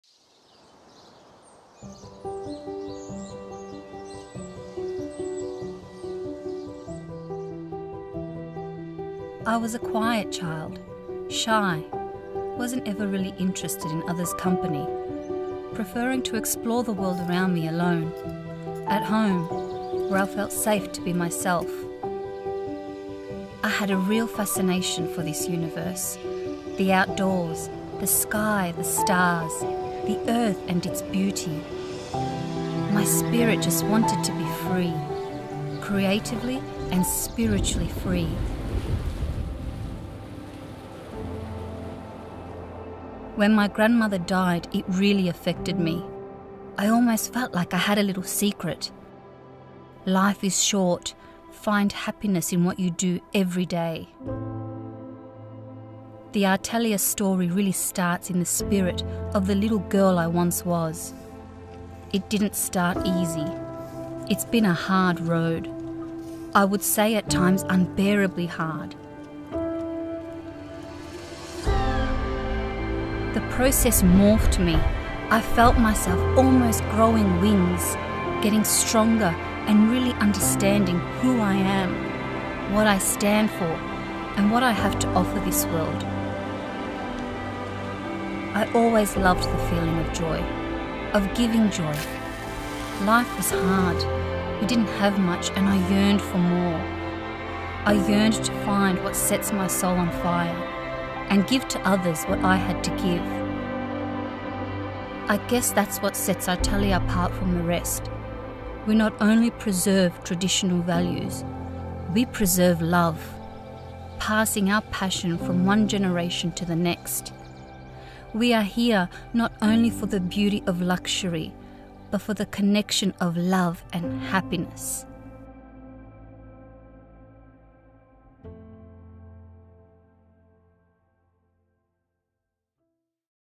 The Artelia Jewellery Story, the drive behind the vision
With bold ambitions and a clear vision, Artelia is a history of passion, love and honesty in the creation of fine jewellery.

A well-established jewellery manufacturer with roots in Istanbul, Turkey, Artelia has been manufacturing bespoke jewellery in Australia since the early 1990s.

Roberto Ulas, proudly the Artelia Founder has been creating jewellery pieces since he was eight years old. He first learnt the craft from his father in Turkey and started working full-time for the business at 15. Roberto has been growing the business including the establishment and operation of the largest jewellery design and manufacturing houses in Australia. This required extensive training including here in Australia and alongside master jewellery craftsmen and pioneers in Germany and Italy.